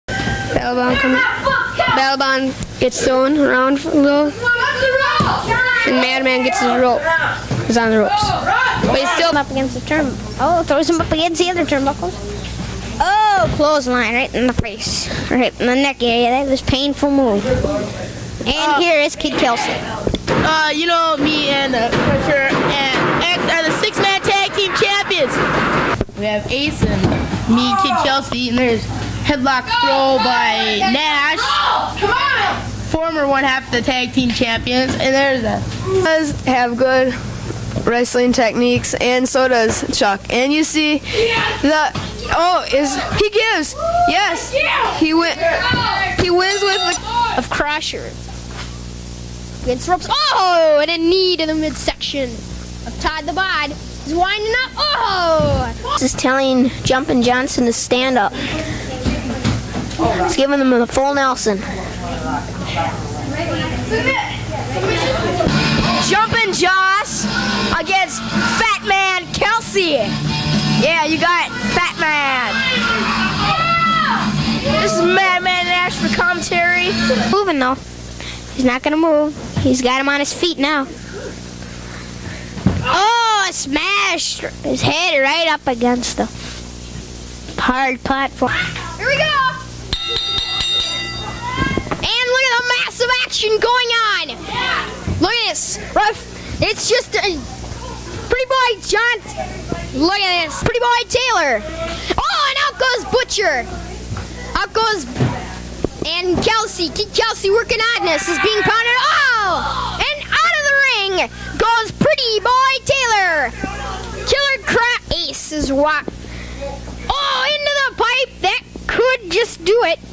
NWF Kids Pro Wrestling Studio Taping 43 Preview
A short preview from some of the matches on Studio Taping 43. The All New NWF Video Network Premium: